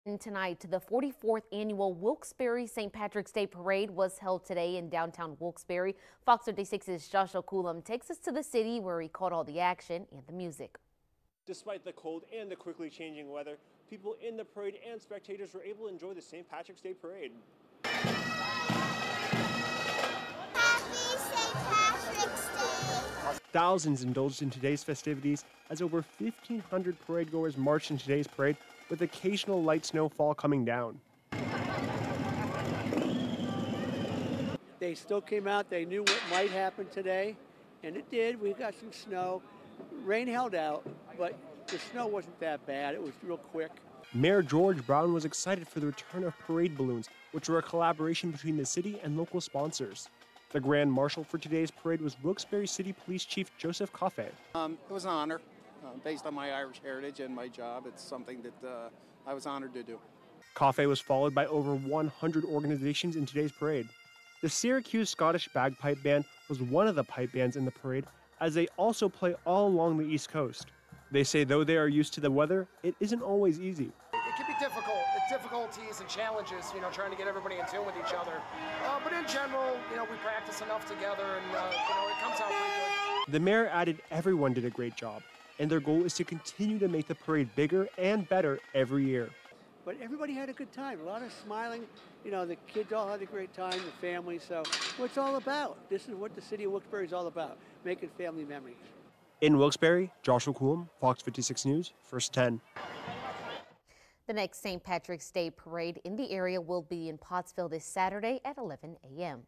Wilkes-Barre enjoys 44th St. Patrick's Day Parade amidst snow
On Sunday the 44th annual Wilkes-Barre’s St. Patrick’s Day Parade was held in downtown Wilkes-Barre.

Despite the cold and the quickly changing weather, people in the parade and spectators were able to join the St. Patrick’s Day Parade.

Thousands indulged in today’s festivities as over fifteen hundred parade goers marched into today’s parade with occasional light snow fall coming down.